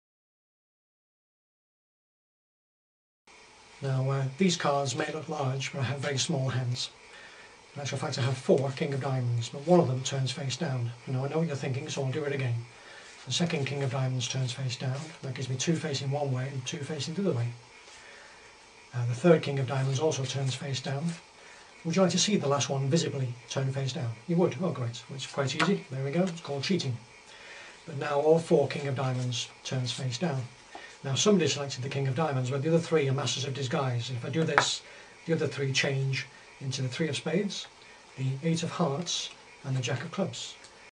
Stephen Tucker's "Quartet" card routine
Four jumbo King of Diamonds magically flip face down one at a time. Then... change into four different cards!